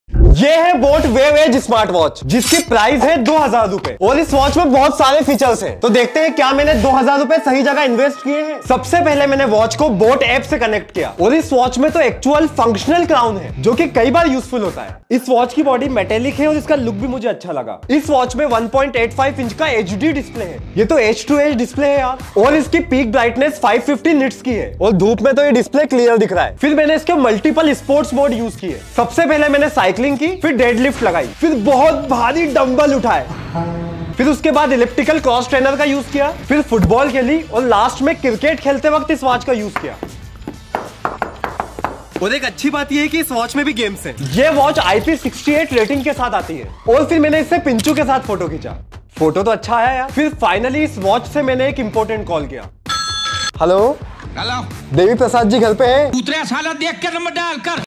Best In Budget Smartwatch? boAt Wave Edge
# paid collab with boAt

boAt Wave Edge Smartwatch price in India is ₹1,999. You can buy boAt Wave Edge Smartwatch online.
Wear your watch in style as you stand out in the crowd. Wave Infinity sports a 1.85” HD display, a 90% screen to body ratio, 550 nits brightness.

Stay charged and ready with up to 7 days of battery backup with eco mode. The IP68 dust and water resistance protects your smartwatch from any and every hazard.